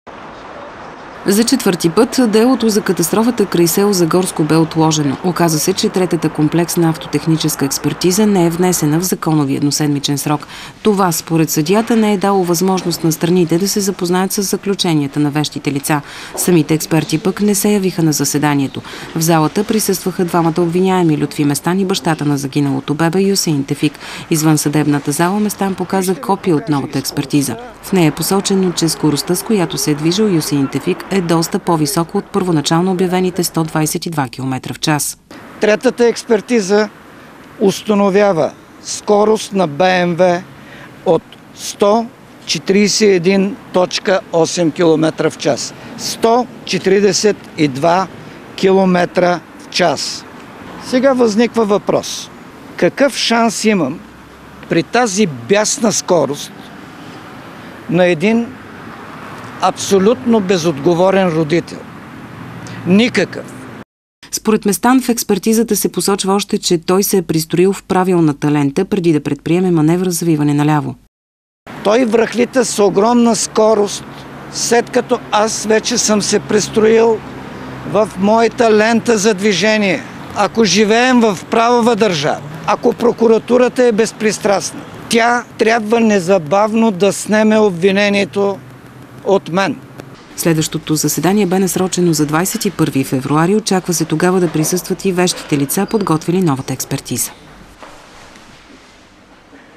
Неявяване на експерти стана причина за поредното отлагане на делото за катастрофата край с. Загорско
Делото за катастрофата край село Загорско, при която загина 6-месечно бебе, отново бе отложено. Причина за това стана неявяване на вещите лица, извършили третата автотехническа експертиза. Нейно копие днес показа Лютви Местан, който е един от обвиняемите.